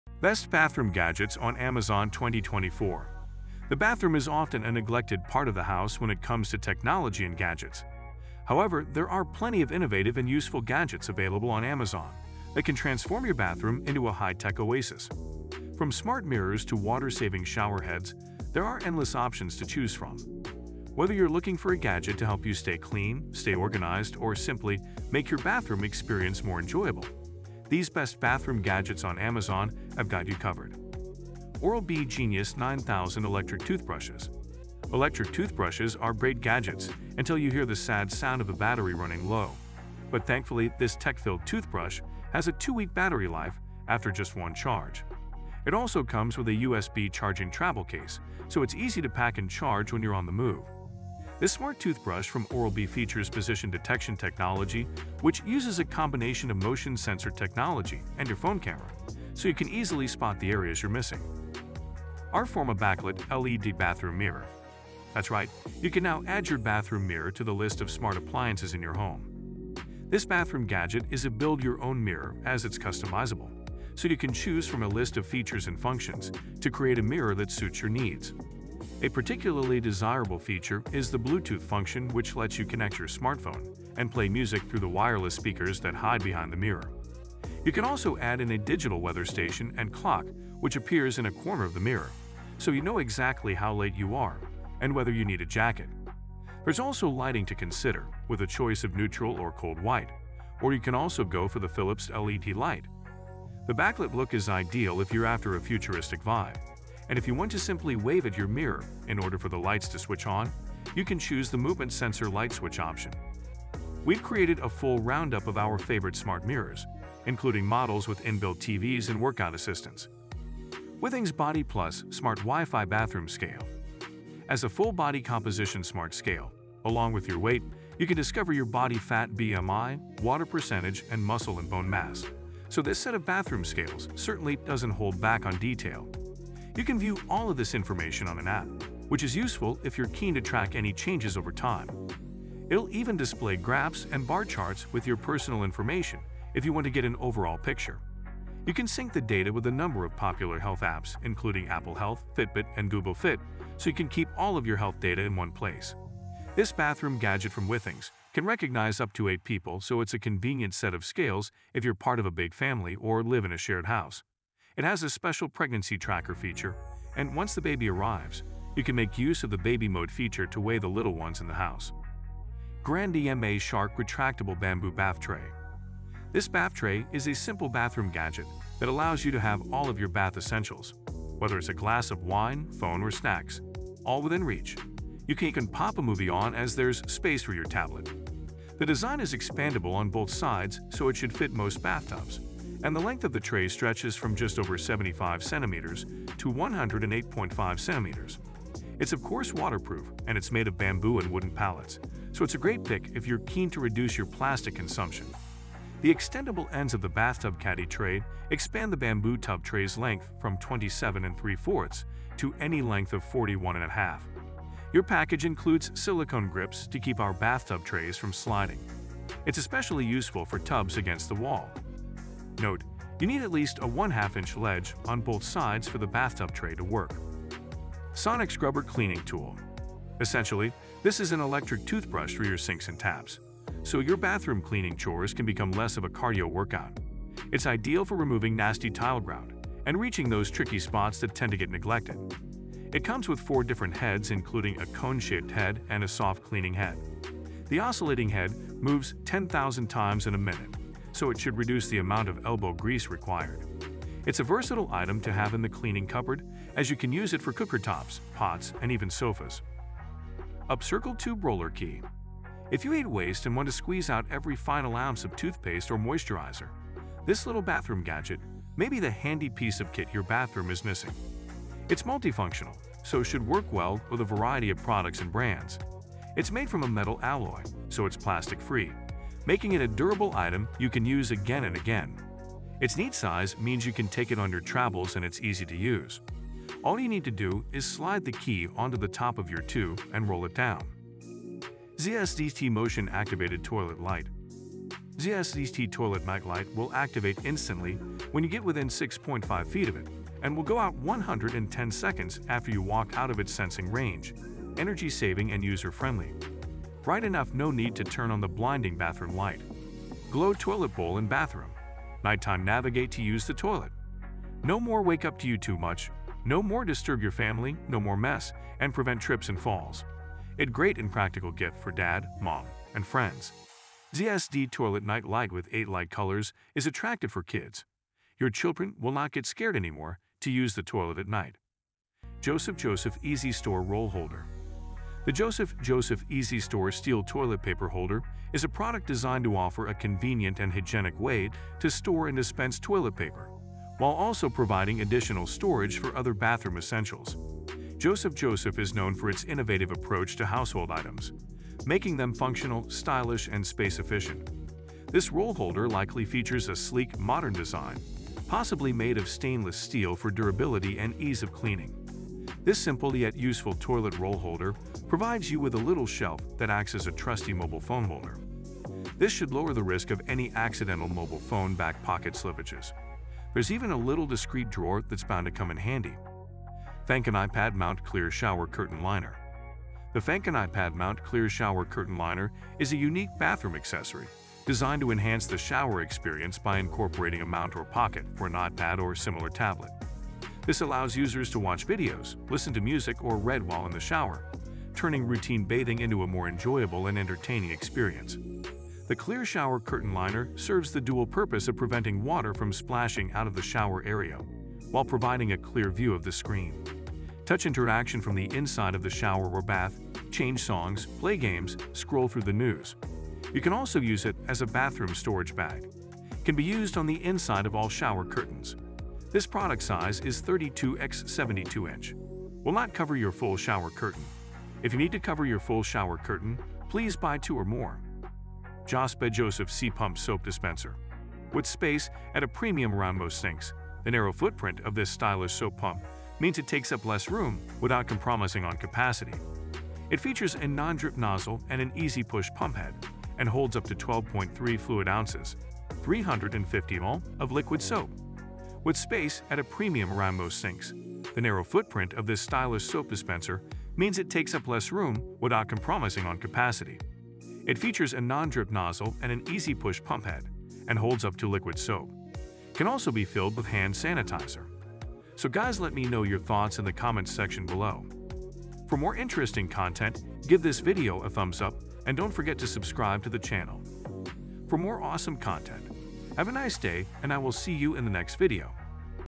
Best Bathroom Gadgets On Amazon 2024
In the ever-evolving world of home technology, the bathroom has become a sanctuary of innovation and convenience. As we step into 2024, Amazon has unveiled an array of gadgets that promise to transform your bathroom experience into something not just functional, but truly exceptional. From smart mirrors that offer weather updates and skincare tips to advanced showerheads that let you bathe in LED lights and your favorite tunes, the future of bathroom technology is here. These gadgets aren't just about adding luxury; they're about enhancing daily routines, promoting wellness, and making those moments of personal care more enjoyable and efficient.